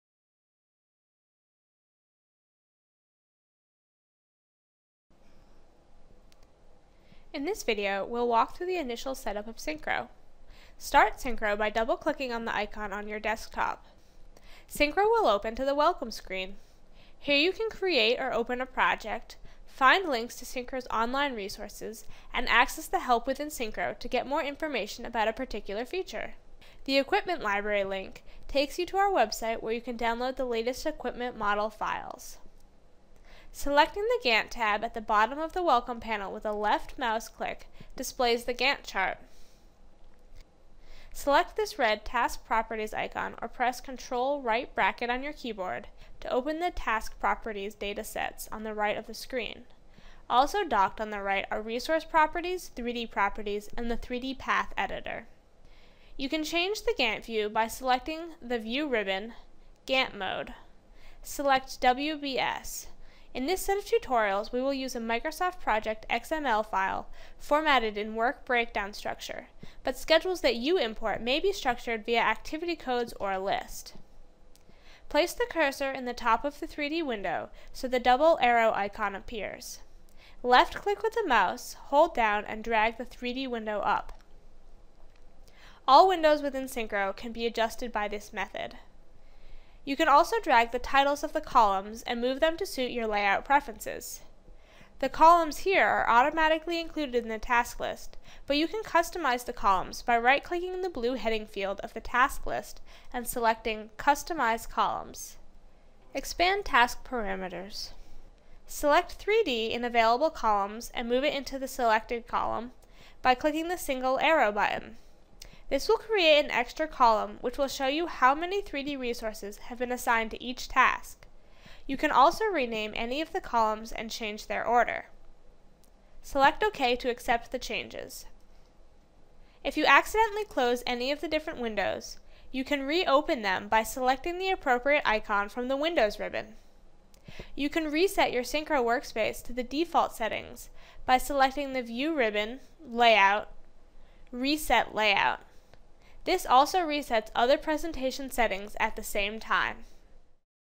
1 - Synchro Training: Workspace
In this video we'll walk through the Initial Setup of Synchro